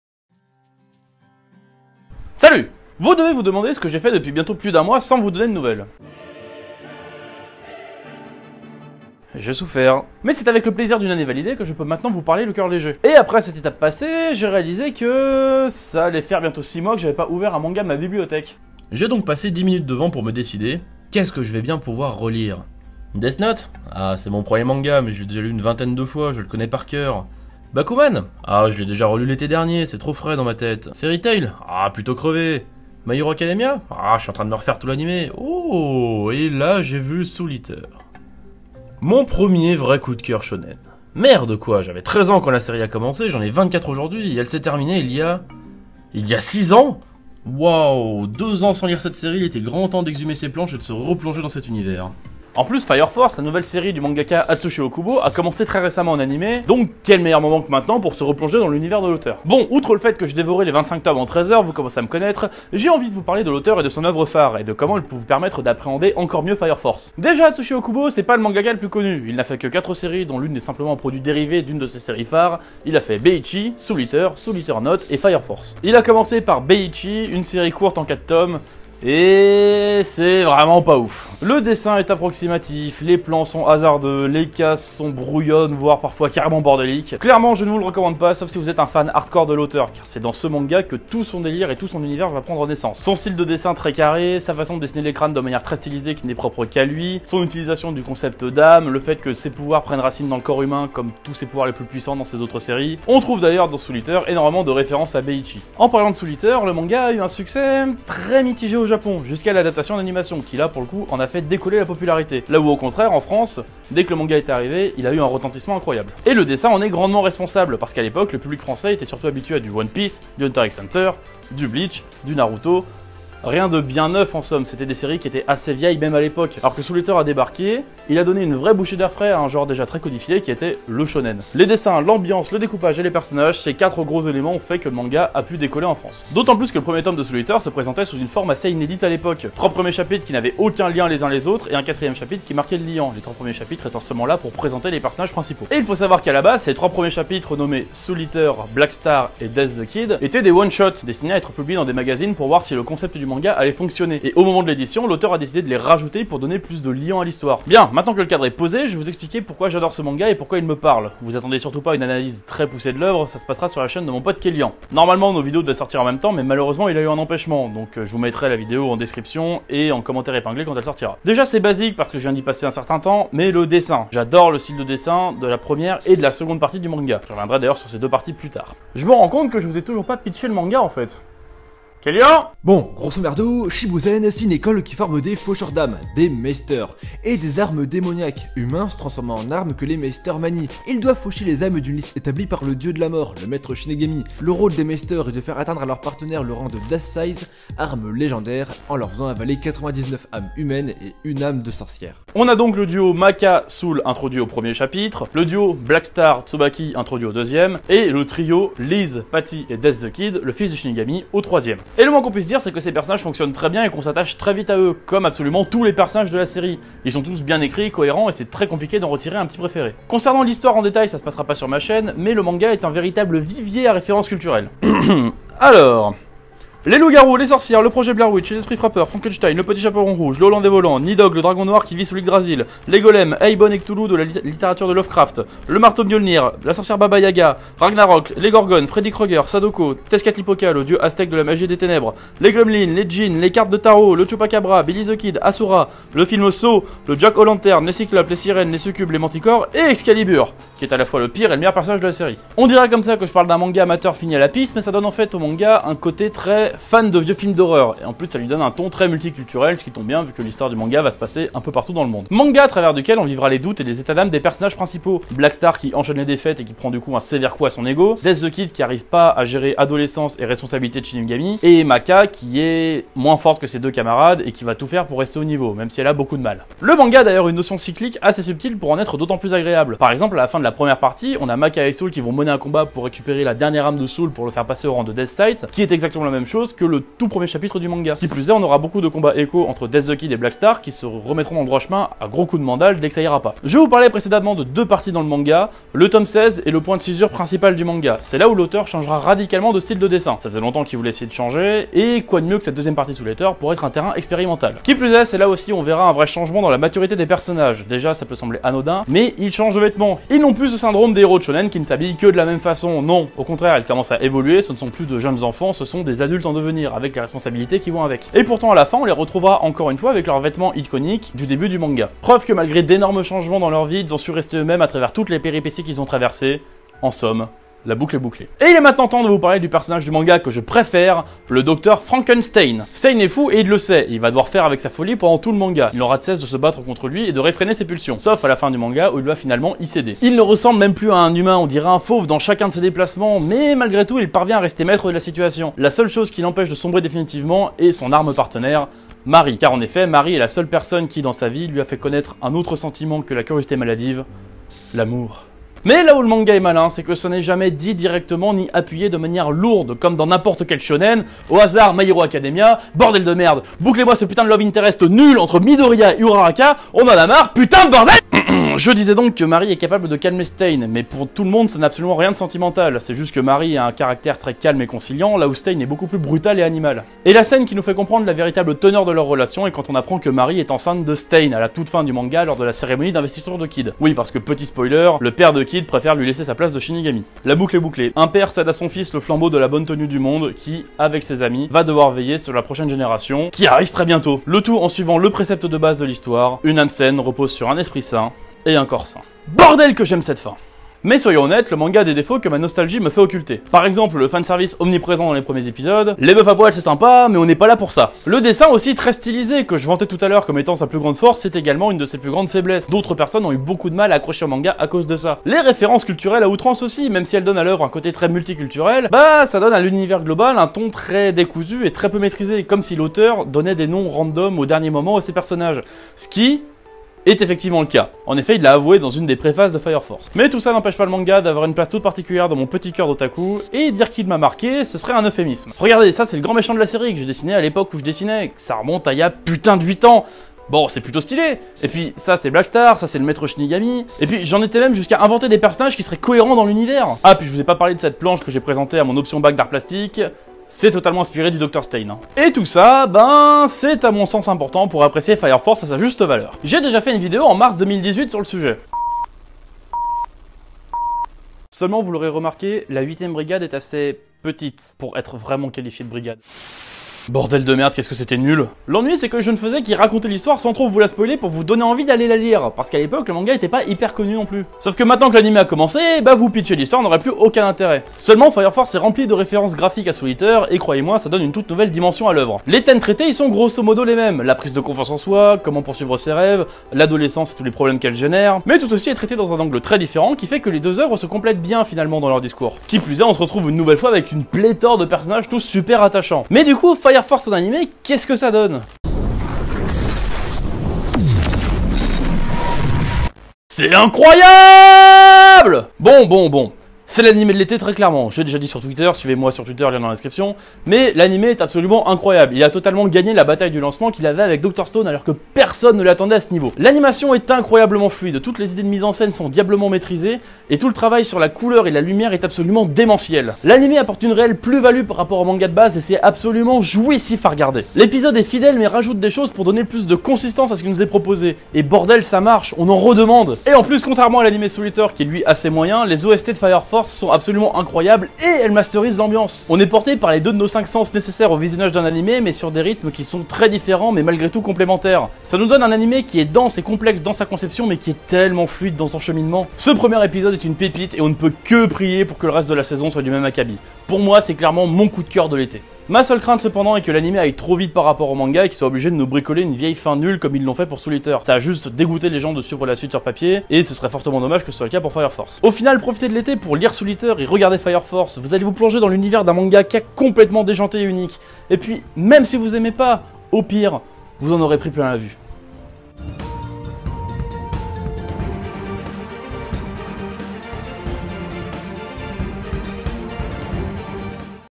FIRE FORCE est excellent ! (le petit frère de SOUL EATER)
ENFIN DE RETOUR ! Vous m'avez manqué bordel !

Merci à Kelyan d'avoir participé à la vidéo ! Sa vidéo sur le sujet arrive bientôt, je vous préviendrais sur les réseaux.

J'ai tenté quelque chose avec la miniature, dites moi ce que vous en pensez.

J'ai commencé à lire Naruto, tenez le vous pour dit ;)